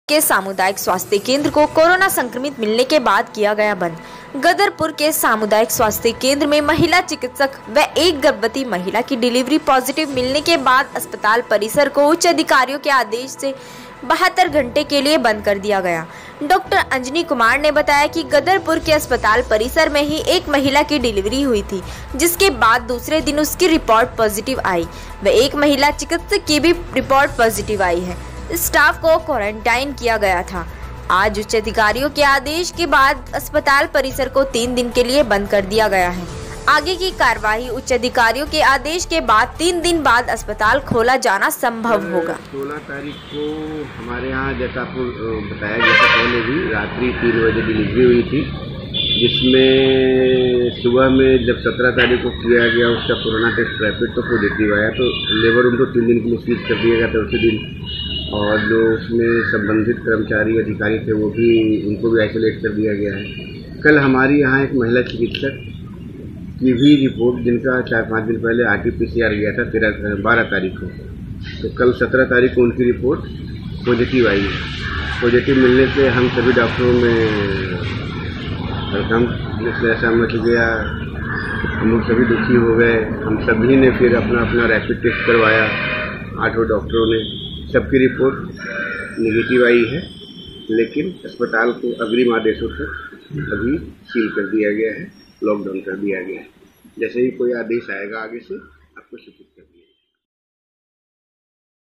गदरपुर के सामुदायिक स्वास्थ्य केंद्र को कोरोनावायरस मिलने के बाद किया गया बंद
स्थान उतराखड गदरपुर
रिपोर्ट वीरेंद्र रावत
एंकर ,,,,गदरपुर के सामुदायिक स्वास्थ्य केंद्र में महिला चिकित्सक व एक गर्भवती महिला की डिलीवरी के बाद पॉजिटिव मिलने के बाद अस्पताल परिसर को उच्च अधिकारियों के आदेश से 72 घंटे के लिए बंद कर दिया गया डॉक्टर अंजनी कुमार ने बताया कि गदरपुर के अस्पताल परिसर में ही एक महिला की डिलीवरी हुई थी जिसके बाद दूसरे दिन सुबह उसकी रिपोर्ट पॉजिटिव आई व एक महिला चिकित्सक की भी रिपोर्ट पॉजिटिव आई है स्टाफ को कैंटीन किया गया था आज उच्च अधिकारियों के आदेश के बाद अस्पताल परिसर को 3 दिन के लिए बंद कर दिया गया है आगे की कार्रवाई उच्च अधिकारियों के आदेश के बाद 3 दिन बाद अस्पताल खोला जाना संभव होगा।

 बाइट डॉक्टर अंजनी कुमार वरिष्ठ चिकित्सक सामुदायिक स्वास्थ्य केंद्र